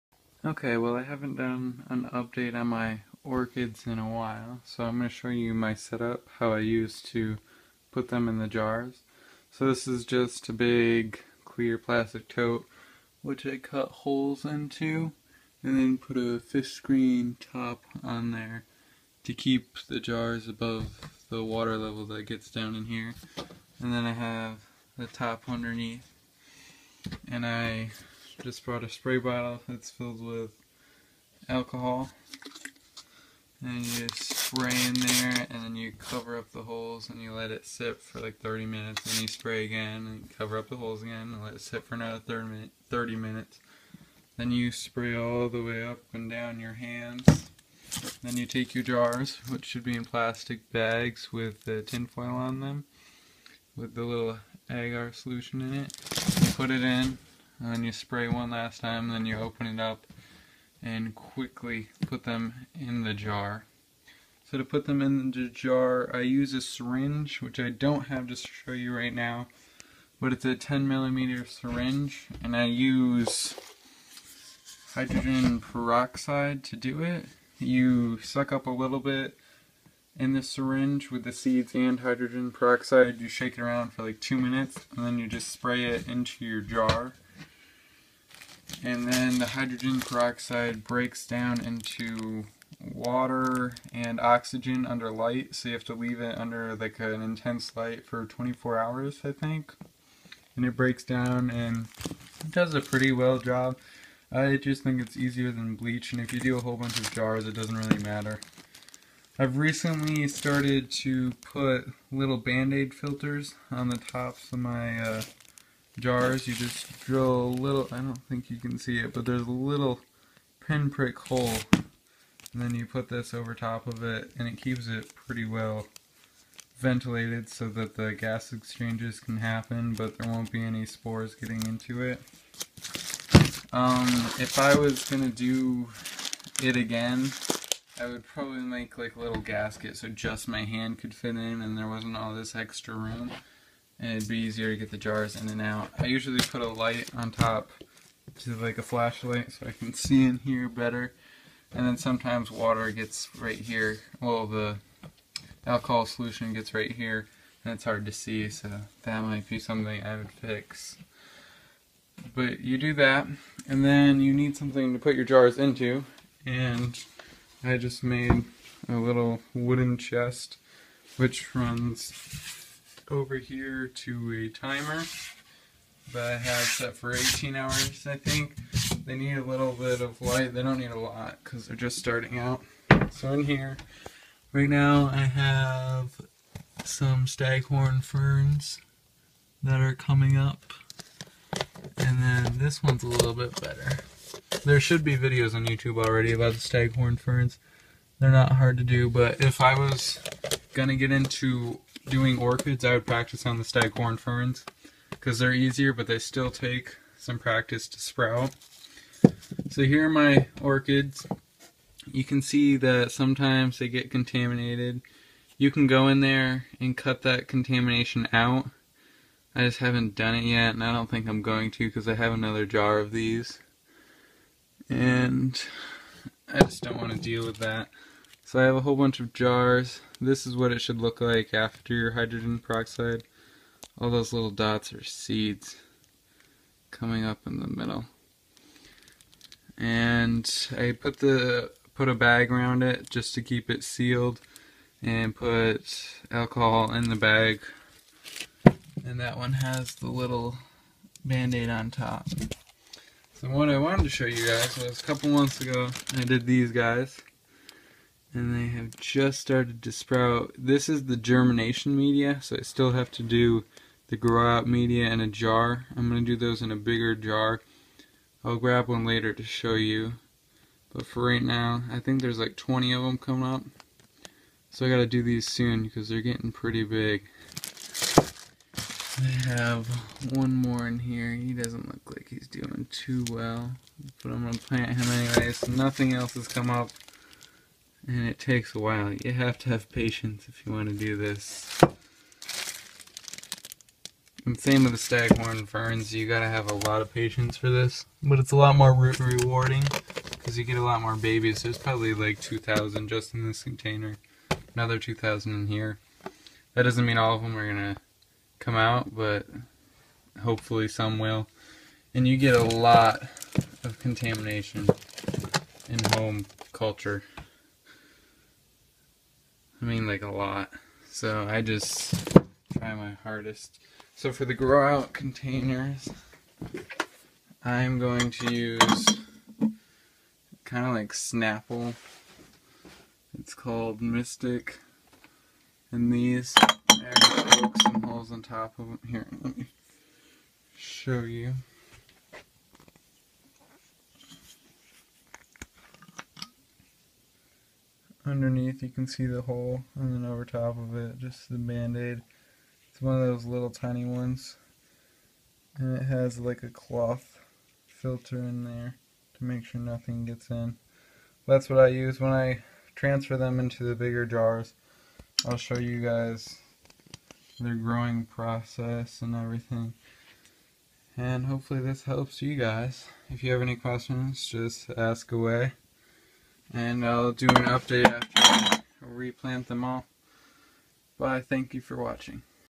Orchid Seed Germination
This is my setup of orchid seed germination when they are still in there first jar.